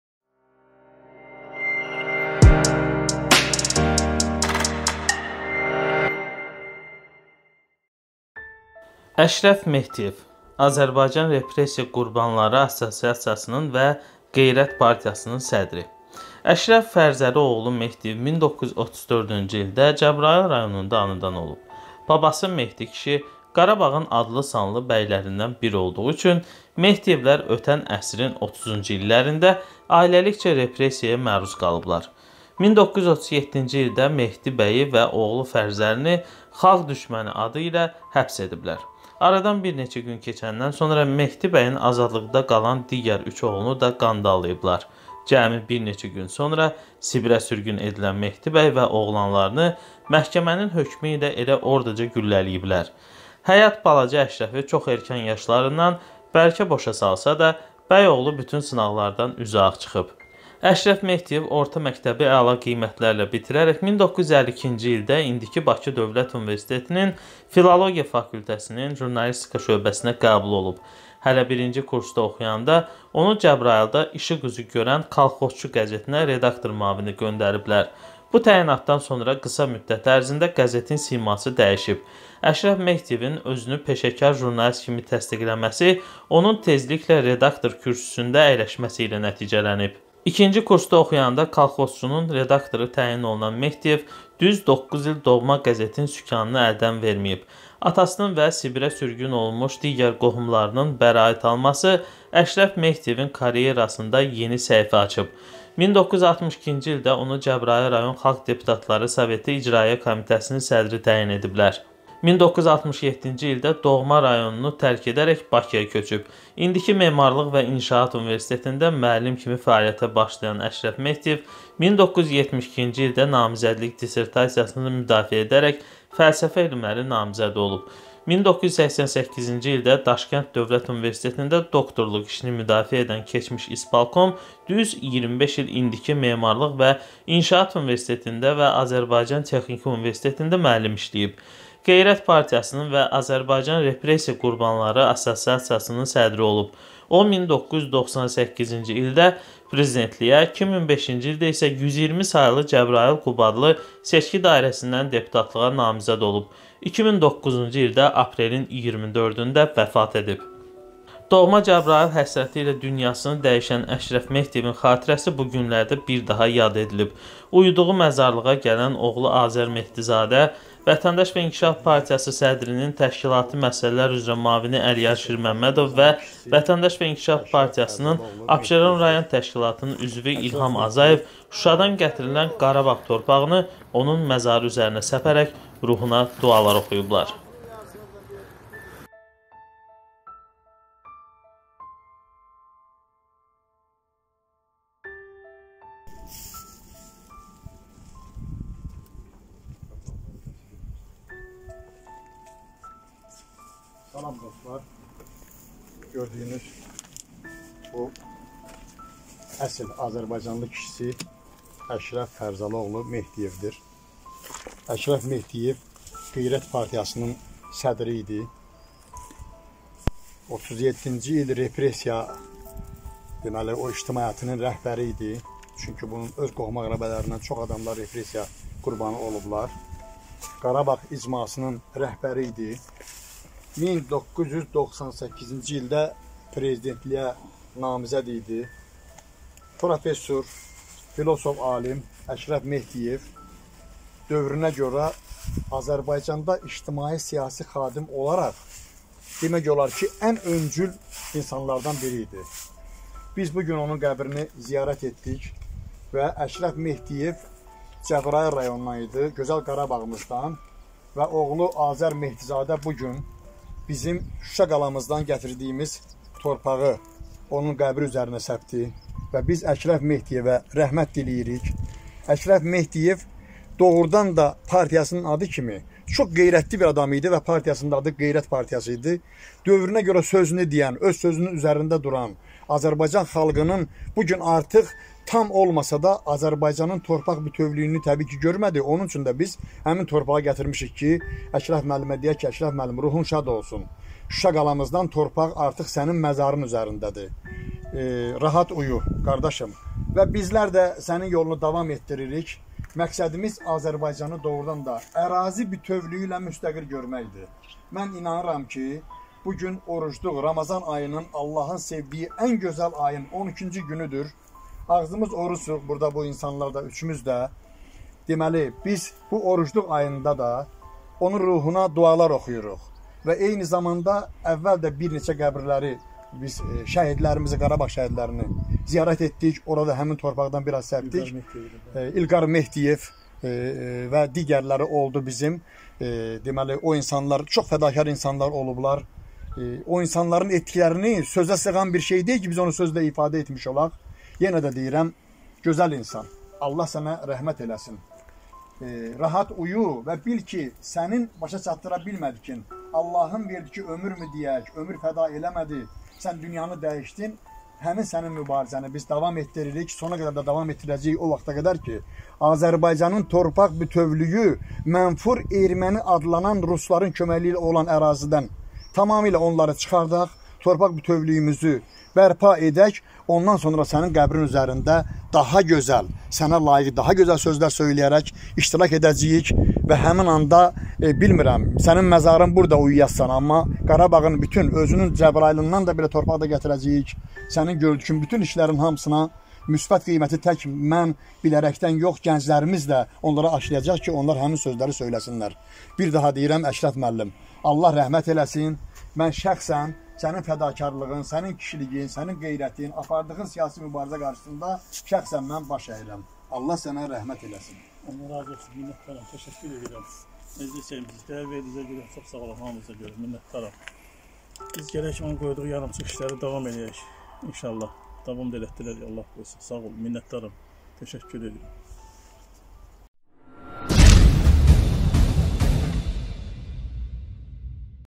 ƏŞRƏF MEHDİYEV.ÖMRÜNÜ MÜBARİZƏYƏ HƏSR EDƏN İNSAN
Əşrəf Mehdiyev - Azərbaycan Repressiya Qurbanları Assosiasiyasının və Qeyrət Partiyasının sədri. Əşrəf Fərzəli oğlu Mehdiyev 1934-cü ildə Cəbrayıl rayonunda anadan olub. Babası Mehdi kişi Qarabağın adlı-sanlı bəylərindən biri olduğu üçün Mehdiyevlər ötən əsrin 30-cu illərində ailəlikcə repressiyaya məruz qalıblar. 1937-ci ildə Mehdi bəyi və oğlu Fərzəlini “xalq düşməni” adı ilə həbs ediblər. Aradan bir neçə gün keçəndən sonra Mehdi bəyin azadlıqda qalan digər 3 oğlunu da qandallayıblar. Cəmi bir neçə gün sonra Sibirə sürgün edilən Mehdi bəy və oğlanlarını məhkəmənin hökmü ilə elə oradaca güllələyiblər. Həyat balaca Əşrəfi çox erkən yaşlarından bərkə-boşa salsa da, bəy oğlu bütün sınaqlardan üzüağ çıxıb. Əşrəf Mehdiyev Orta məktəbi əla qiymətlərlə bitirərək, 1952-ci ildə indiki BDU-nun filologiya fakültəsinin jurnalistika şöbəsinə qəbul olub. Hələ birinci kursda oxuyanda onu Cəbrayılda işıq üzü görən “Kolxozçu” qəzetinə redaktor müavini göndəriblər. Bu təyinatdan sonra qısa müddət ərzində qəzetin siması dəyişib. Əşrəf Mehdiyevin özünü peşəkar jurnalist kimi təsdiqləməsi onun tezliklə redaktor kürsüsündə əyləşməsi ilə nəticələnib. İkinci kursda oxuyanda “Kolxozçu”nun redaktoru təyin olunan Mehdiyev düz 9 il doğma qəzetin sükanını əldən verməyib.Atasının və Sibirə sürgün olunmuş digər qohumlarının bəraət alması Əşrəf Mehdiyevin karyerasında yeni səhifə açıb. 1962-ci ildə onu Cəbrayıl rayon Xalq Deputatları Soveti İcraiyyə Komitəsinin sədri təyin ediblər. 1967-ci ildə doğma rayonunu tərk edərək, Bakıya köçüb. İndiki Memarlıq və İnşaat Universitetində müəllim kimi fəaliyyətə başlayan Əşrəf Mehdiyev 1972-ci ildə namizədlik dissertasiyasını müdafiə edərək, fəlsəfə elmləri namizədi olub. 1988-ci ildə Daşkənd Dövlət Universitetində doktorluq işini müdafiə edən keçmiş “ispalkom” düz 25 il indiki Memarlıq və İnşaat Universitetində və Azərbaycan Texniki Universitetində müəllim işləyib. Qeyrət Partiyasının və Azərbaycan Repressiya Qurbanları Assosiasiyasının sədri idi. O, 1998-ci ildə prezidentliyə, 2005-ci ildə isə 120 saylı Cəbrayıl-Qubadlı Seçki Dairəsindən deputatlığa namizəd olub.
2009-cu ildə aprelin 24-də vəfat edib.

Doğma Cəbrayıl həsrəti ilə dünyasını dəyişən Əşrəf Mehdiyevin xatirəsi bu günlərdə bir daha yad edilib.Uyuduğu məzarlığa gələn oğlu Azər Mehdizadə,Vətəndaş və İnkişaf Partiyası sədrinin təşkilati məsələlər üzrə müavini Əliyar Şirməmmədov və Vətəndaş və İnkişaf Partiyasının Abşeron rayon təşkilatının üzvü İlham Azayev Şuşadan gətirilən Qarabağ torpağını onun məzarı üzərinə səpib ruhuna dualar oxuyublar